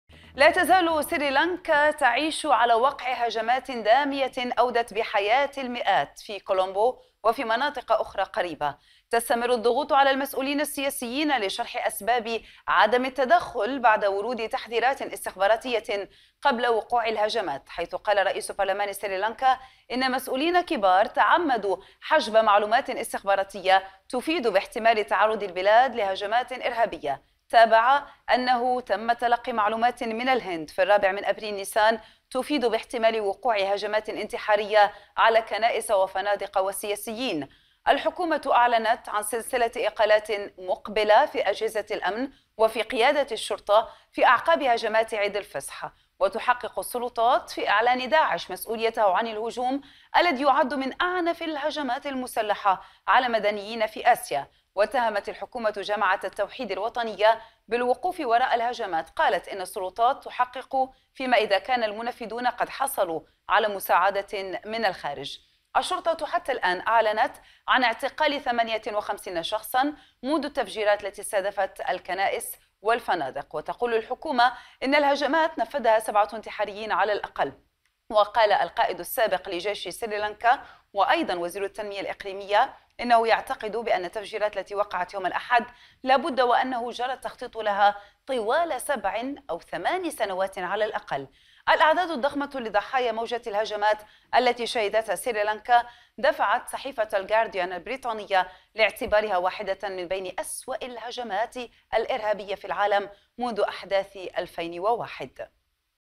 سريلانكا الحكومة تعلن عن سلسلة إقالات قريبة
أخبار الآن | كولومبو - سريلانكا (أ ف ب)

لا تزال سريلانكا تعيش على وقع هجمات دامية أودت بحياة المئات، الأحد، في كولومبو ومناطق أخرى قريبة.

تستمر الضغوط على المسؤولين السياسيين لشرح أسباب عدم التدخل بعد ورود تحذيرات استخباراتية قبل وقوع الهجمات.

حيث قال رئيس برلمان سريلانكا، إن مسؤولين كباراً تعمدوا حجب معلومات استخباراتية تفيد باحتمال تعرض البلاد لهجمات إرهابية، وتابع أنه تم تلقي معلومات من الهند في الرابع من أبريل/نيسان تفيد باحتمال وقوع هجمات انتحارية على كنائس وفنادق وسياسيين

تحقق السلطات في إعلان داعش مسؤوليته عن الهجوم الذي يعد من أعنف الهجمات المسلحة على مدنيين في آسيا.

واتهمت الحكومة "جماعة التوحيد الوطنية" بالوقوف وراء الهجمات، وقالت إن السلطات تحقق فيما إذا كان المنفذون قد حصلوا على مساعدة من الخارج.

وأعلنت الشرطة حتى الآن عن اعتقال 58 شخصا منذ التفجيرات التي استهدفت ثلاث كنائس وثلاث فنادق.

وتقول الحكومة إن الهجمات نفذها 7 انتحاريين على الأقل.

وقال ساراث فونيسكا القائد السابق لجيش سريلانكا ووزير التنمية الإقليمية إنه يعتقد أن التفجيرات التي وقعت الأحد لا بد وأنه جرى التخطيط لها طوال سبع أو ثماني سنوات على الأقل.

الأعداد الضخمة لضحايا موجة الهجمات التي شهدتها سريلانكا، الأحد، دفعت صحيفة "الغارديان" البريطانية، لاعتبارها واحدة من بين أسوأ الهجمات الإرهابية في العالم منذ أحداث 2001.